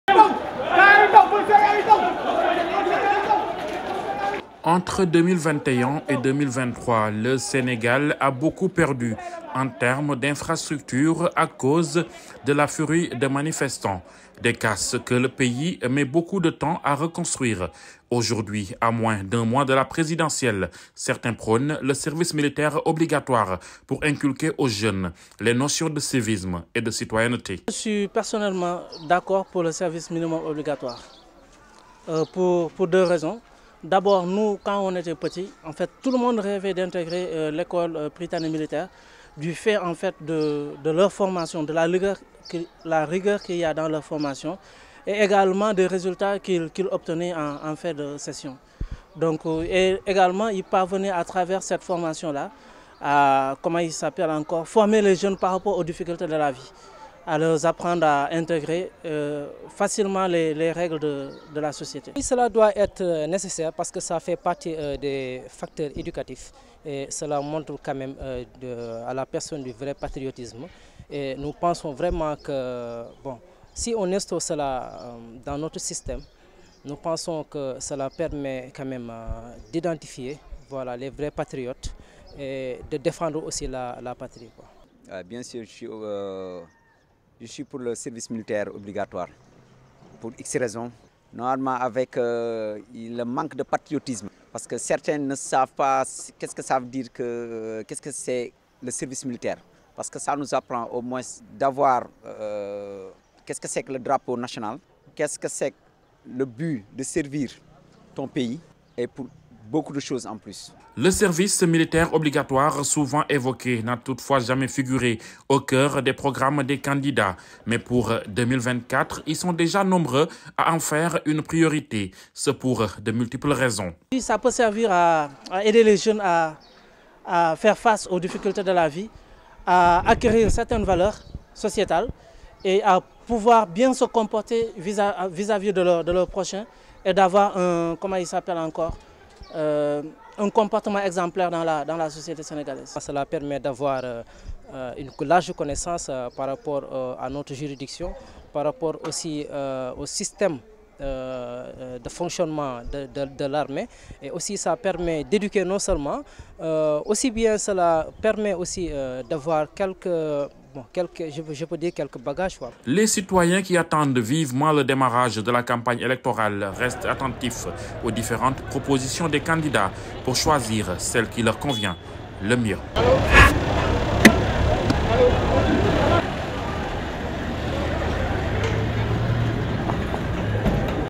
Sénégal: présidentielle, le service militaire obligatoire parmi les propositions des candidats
Entre 2021 et 2023, les différentes manifestations politiques et émeutes ont occasionné d’importants dégâts avec des saccages qui n’ont pas épargné les universités. Des casses que le pays met beaucoup de temps à reconstruire. Aujourd’hui, à moins d’un mois de la présidentielle, certains candidats prônent le service militaire obligatoire pour inculquer aux jeunes les notions de civisme et de citoyenneté.